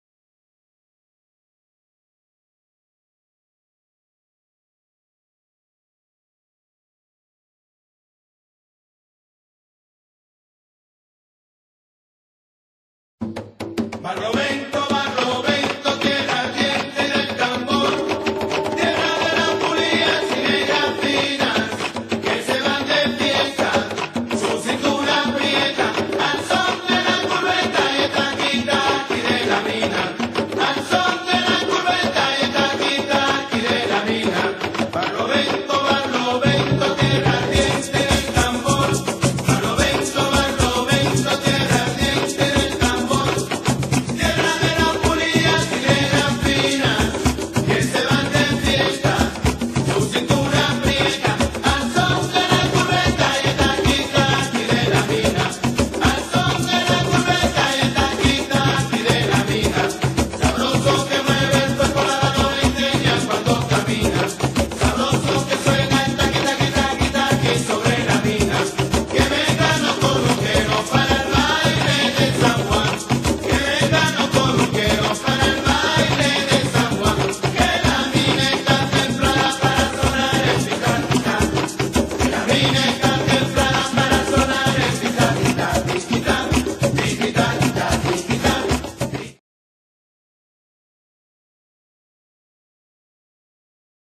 GOLES Orsomarso SC (1) VS Barlovento FC (2) | Copa Telepacífico | Junio 18 de 2018
Cancha Águila Roja | Tercera División | Fecha 12